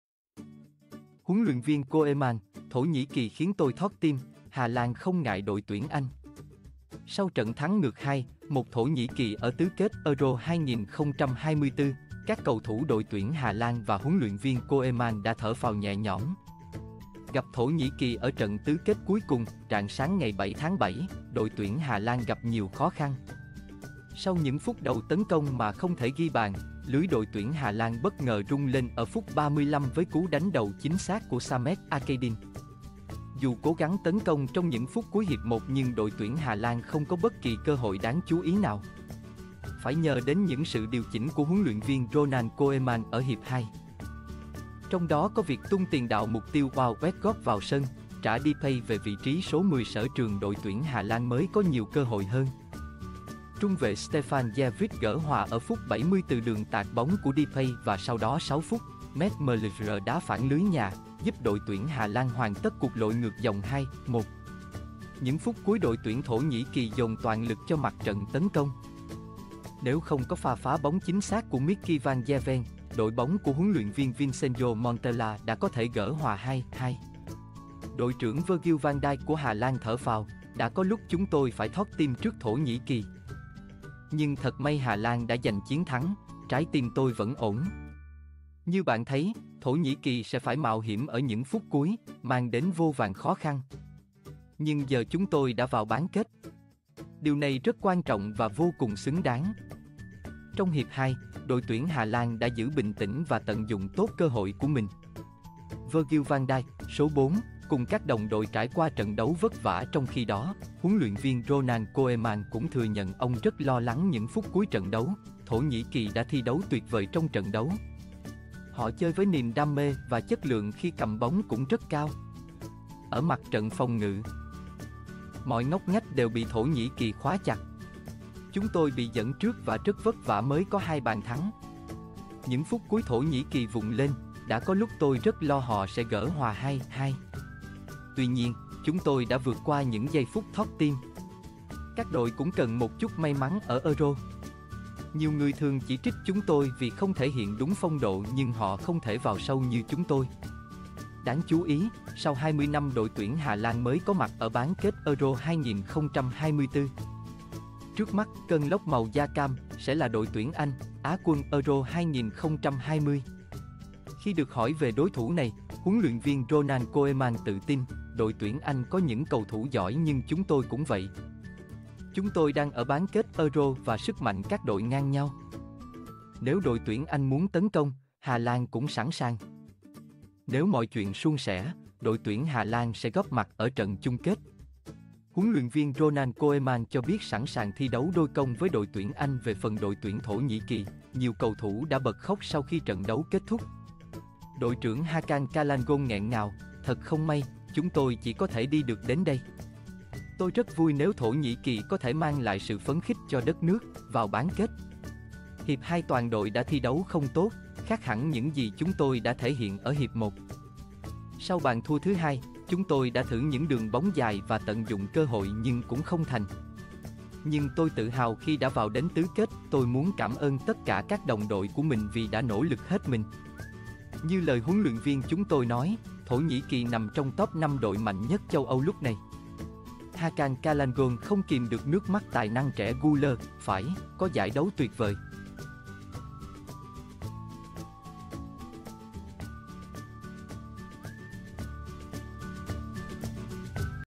HLV Koeman: Thổ Nhĩ Kỳ khiến tôi thót tim, Hà Lan không ngại đội tuyển Anh
Sau trận thắng ngược 2-1 Thổ Nhĩ Kỳ ở tứ kết EURO 2024, các cầu thủ đội tuyển Hà Lan và HLV Koeman đã thở phào nhẹ nhõm.